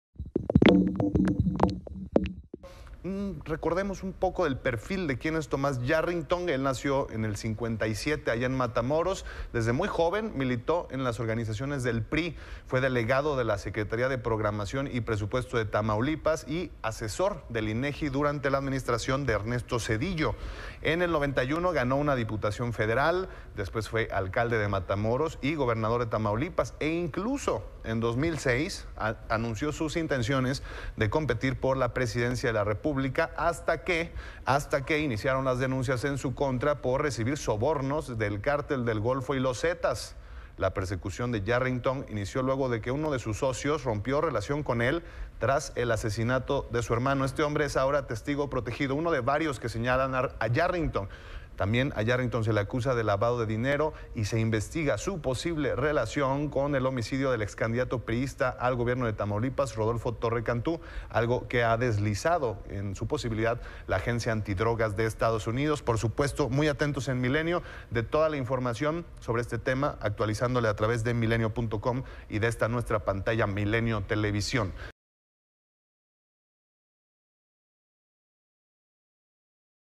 ¿Quién es Tomás Yarrington?
Desde muy joven militó en las organizaciones del PRI, fue asesor del INEGI en la administración de Ernesto Zedillo.

Después fue Alcalde de Matamoros y Gobernador de Tamaulipas.